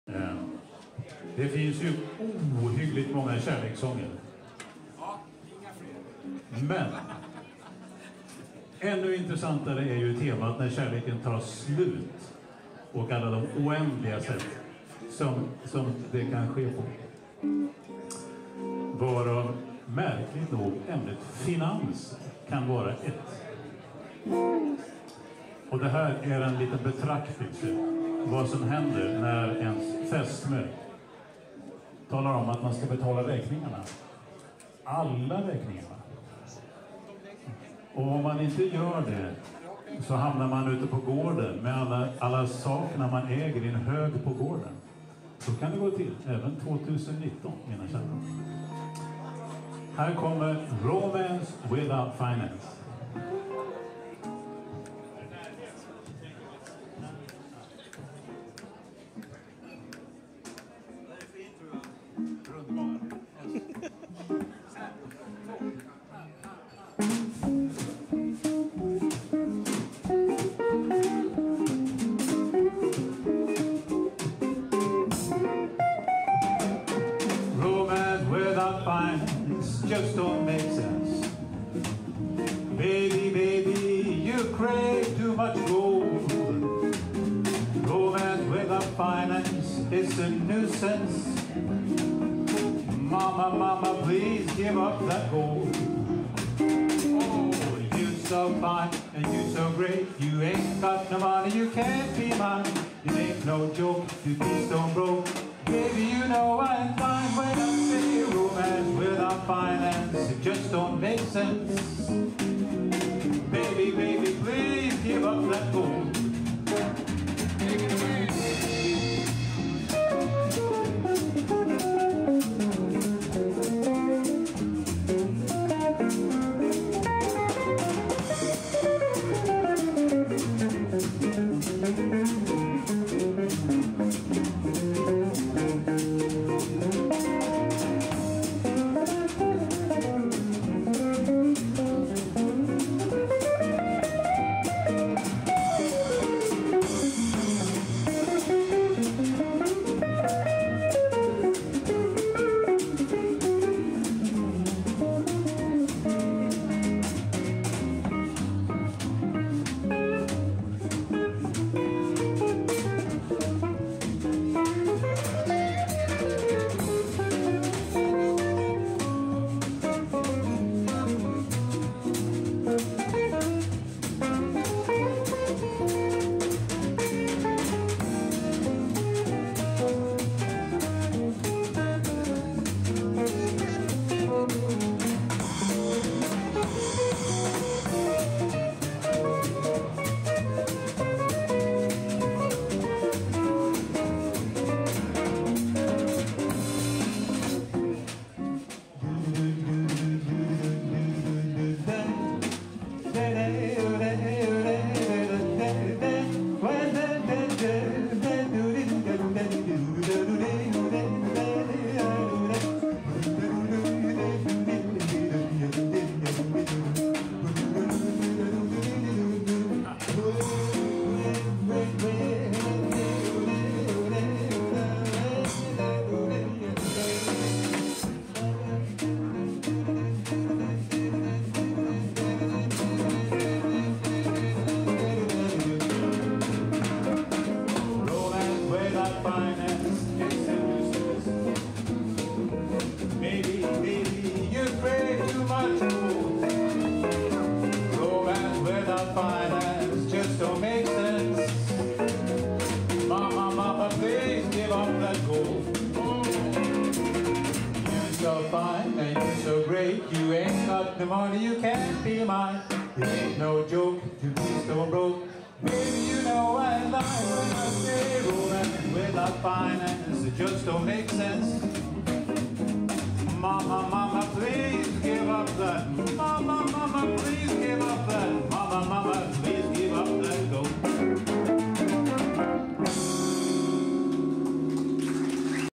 2019-10-19. Två Gitarrer, Katalins lördagsjazz, Uppsala.
Ännu en mysig eftermiddag när de två skickliga gitarristerna Anders Johansson vs. Thomas Arnesen drabbar samman tight groove med Magnus Peterson bas och Håkan Jansson trummor.